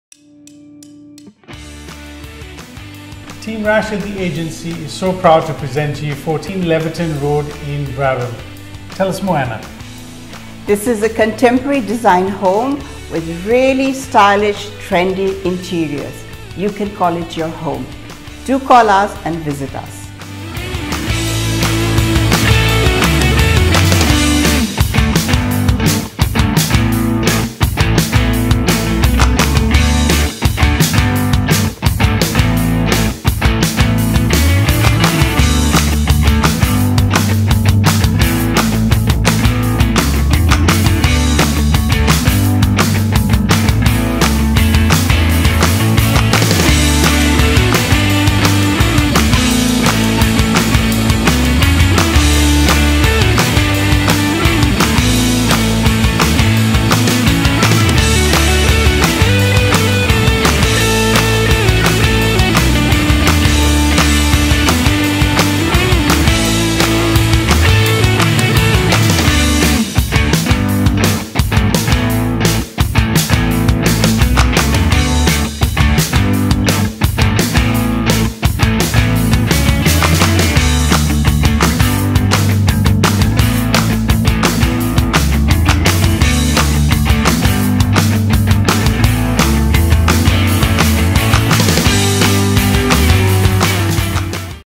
Team Rash Presents 14 Laverton Rd, Brabham
CONTEMPORARY & TRENDY STYLE HOME

This home is finished to reflect trendy and neutral interiors, just like you see in a magazine.

Freshly painted throughout and stylish choice of tiling in common area make this home exceptional.
This home is an expression of WoW!!. that’s the feeling.

JUST 5 MINUTES WALKING DISTANCE TO BRABHAM PRIMARY SCHOOL!!! BIG BONUS

The design of the house offers multiple areas for entertainment.
• Theatre room offers indoor multi-media entertainment.
• Open plan design of kitchen, dining, and family for quality time with family.
• Games room for a game of pool with buddies and lots of fun.
• Large alfresco area under the main roof for your outdoor entertainment for those
        beautiful long evenings.

Perfect choice of a family home in a modern environment with new developments for shopping and schools. Easy and low maintenances property allowing fewer household chores, more time to relax and enjoy. Built in approximately in 2016, here is your chance to enjoy all the modern comforts of a beautiful home. Three bedrooms and two bathrooms in total includes master bedroom with ensiute and walk-in. You will love the kitchen design with 900mm good quality appliances and a spacious walk –in pantry. Kitchen is positioned perfectly, as daily routine revolves around the kitchen, you will enjoy communicating with the family while cooking. Property comes with 22 solar panels 5Kw saving you energy bills and ducted refrigerated reverse cycle air condition with heating and cooling giving you comfort throughout the year.
It is a place to call it HOME.

Solid buys include:
• Double lockable  garage with internal access
• Neat and tidy street front in a nice neighbourhood
• Generous size master bedroom with ensuite and walk-in-robe
• Two other minor bedrooms come with built-in-robes
• Main bathroom with modern finish and separate functional laundry
• A well-appointed kitchen with storage, 900mm stainless steel oven, gas cooktop and
        a spacious walk-in pantry.
• Dining area overlooking the large covered alfresco area
• Family area is stylishly decorated and perfect setting to relax  with the family
• Separate theatre room at the entrance to entertain your guests
• Separate games room opening to the Alfresco area for buddy time
• Large Alfresco under main roof for nice outdoors fun time
• Cubby house and trampoline at the backyard for kids’ play time
•       22 solar panels 5Kw for eco-conscious buyers to save electricity bills
•       Ducted air conditioning with reverse cycle with zonings for comfort
• Easy care gardens with small lawn and shady plants add to the beauty of this house
• Year built: 2016 approximately
• Block size: 373 sqm approximately
• Total built area: 228 sqm approximately
• Builder: Commodore Homes
• Shire rates: $2013 approx. per annum
• Water rates:
Close to:
• Great local outdoor community parks like Watervalley Boulevard park, Sinopia Park,
         Basketball Park and Whiteman Edge kickout Oval
• Renown Swan valley with internal cuisine and fine wine. Your weekends will never be
        boring
• Caversham Park and Whiteman Park to experience an Australian wildlife outdoors.
Easy access to:
• Newly developed infrastructure of Lord Street, Reid highway and new Muchea Link.
• 20 minutes’ drive to Perth Airport and 25 minutes’ drive to Perth CBD

If you are looking for a spacious and well- planned home with low maintenance, then this is for you.
A must-see home.

Why wait!!! Call Today!!!
THE AGENCY    THE EXPERTISE   THE RESULTS